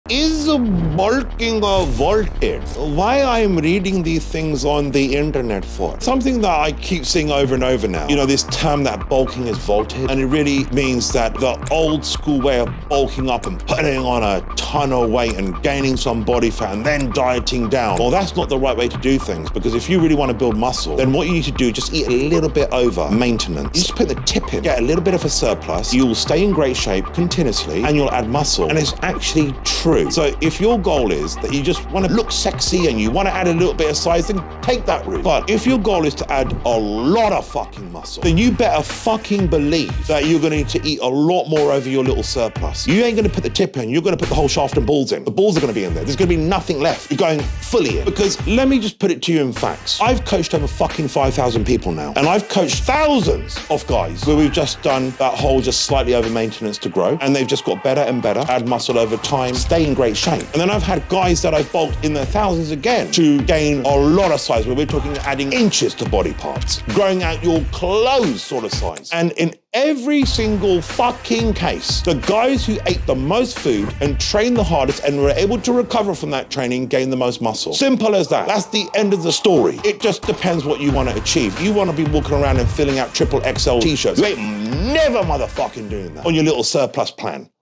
Bulking vs. 'Maingaining': Science vs. Reality
Does bulking even work?
Is 'maingaining' the way?